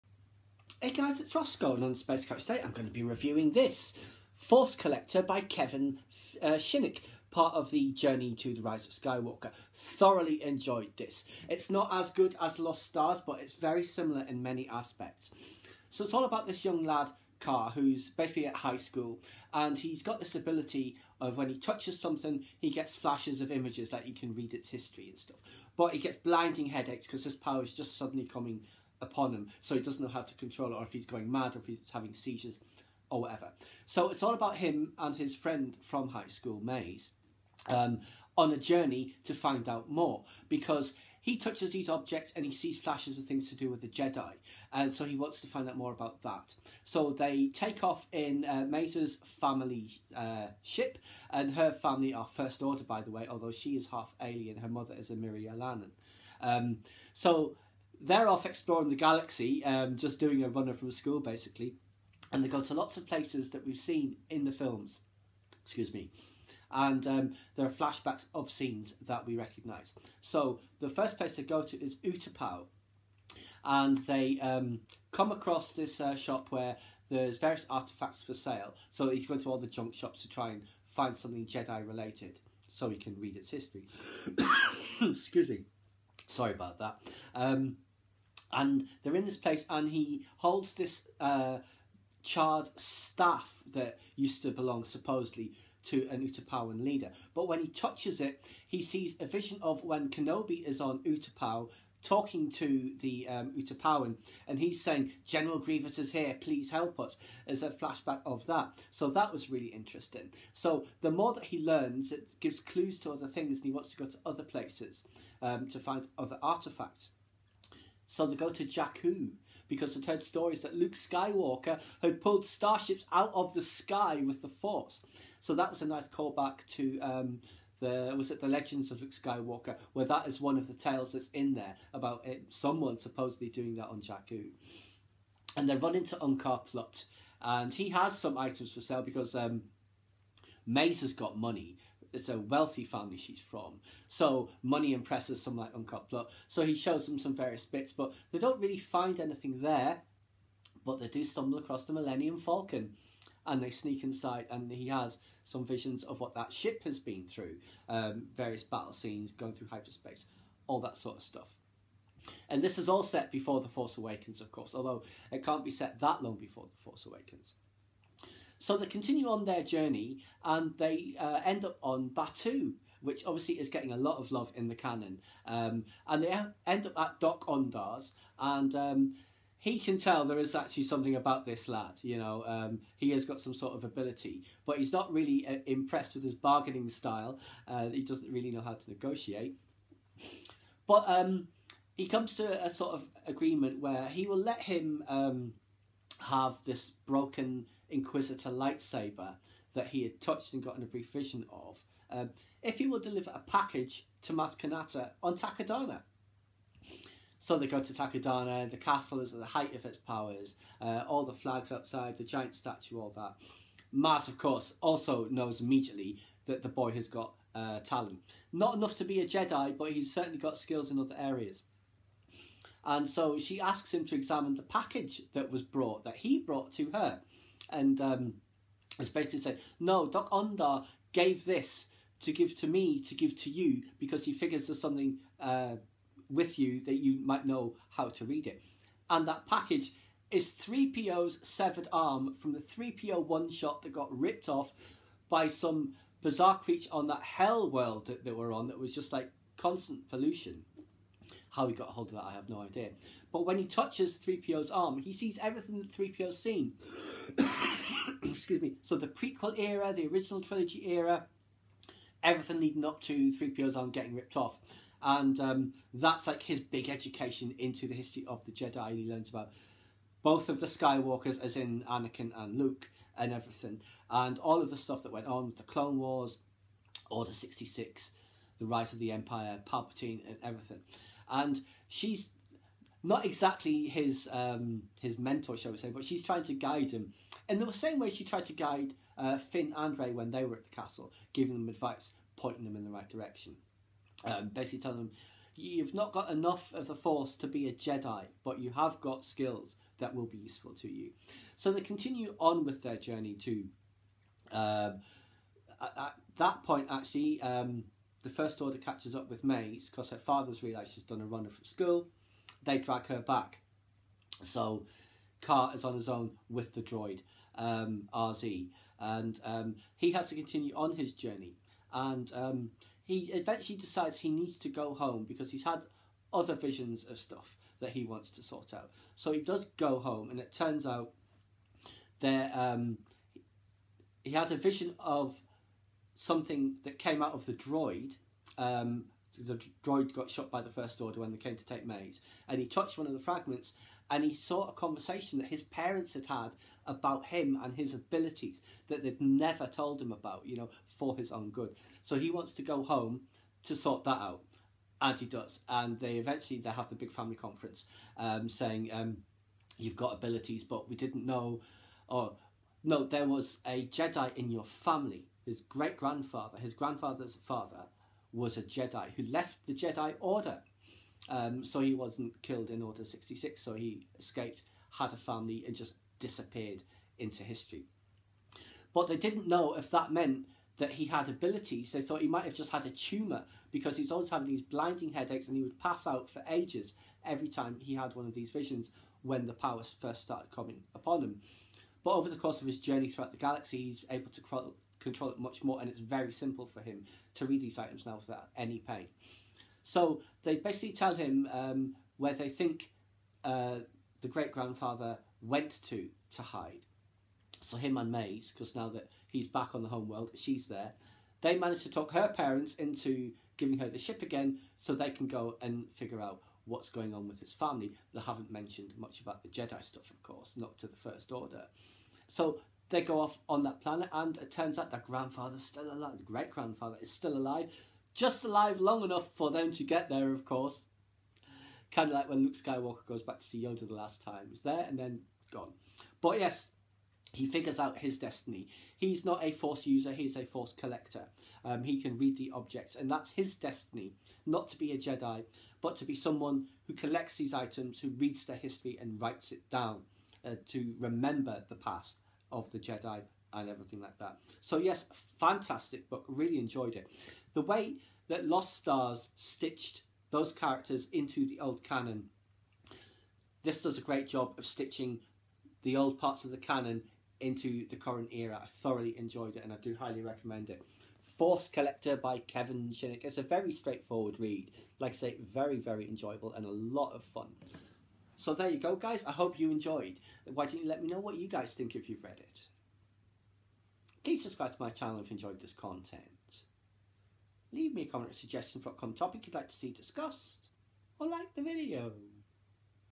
Force Collector by Kevin Shinick - a Space Couch book review!
Thoroughly enjoyed this, although how it will tie in to TROS is anyone’s guess 😊